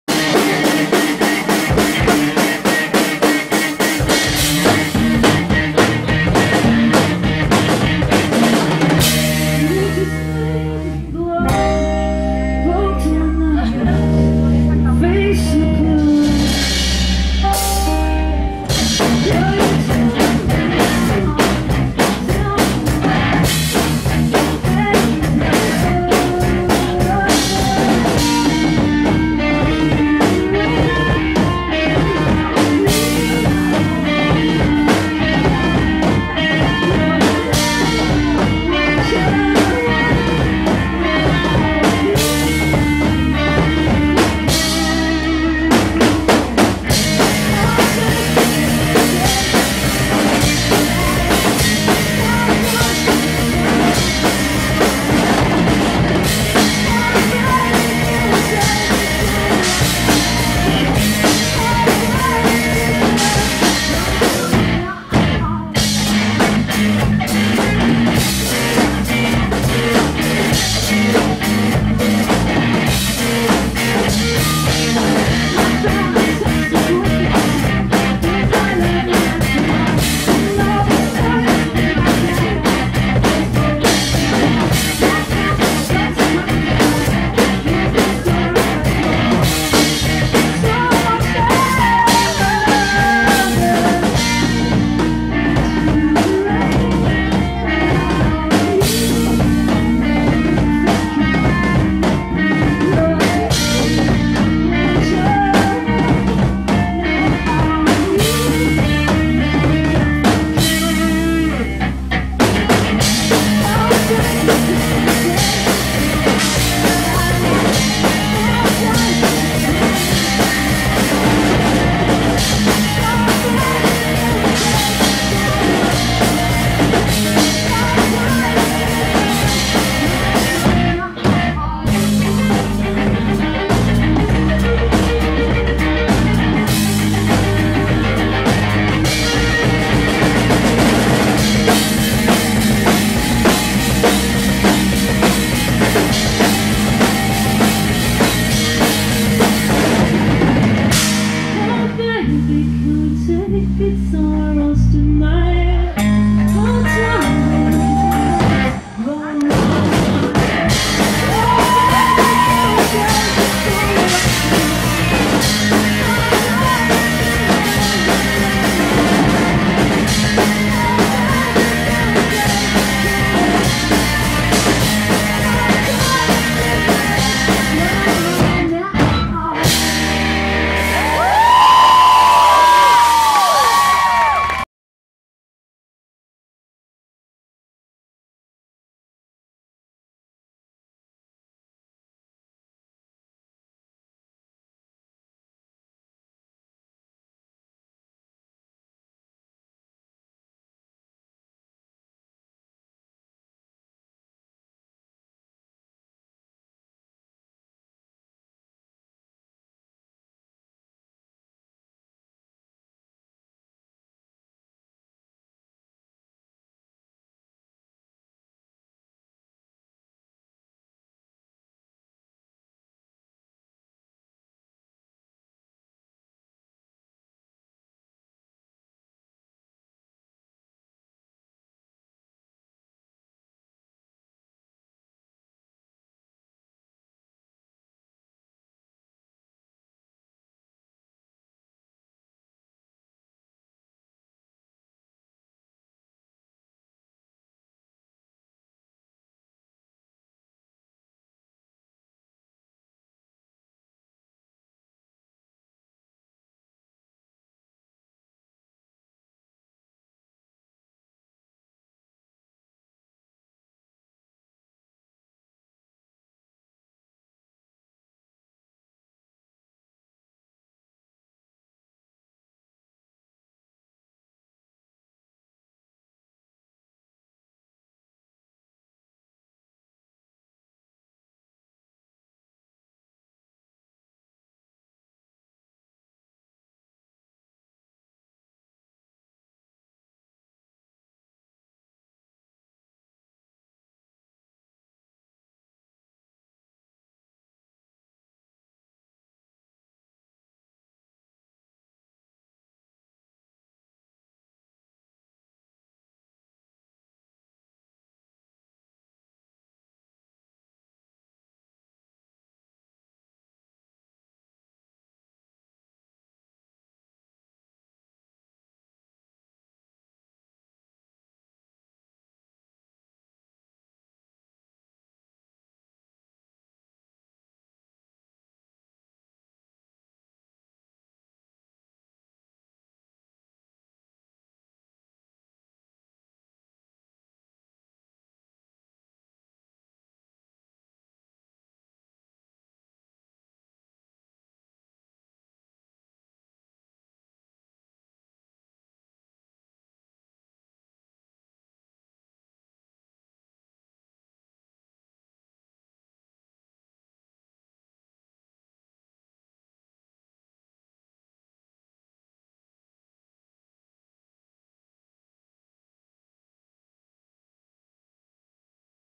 Ninah Mars and the Stickfaces - New Song!
Toque de Ninah Mars and the Stickfaces en el local KapitalCcs el domingo 17 de Julio de 2011 con uno de sus nuevos temas. Visita mi blog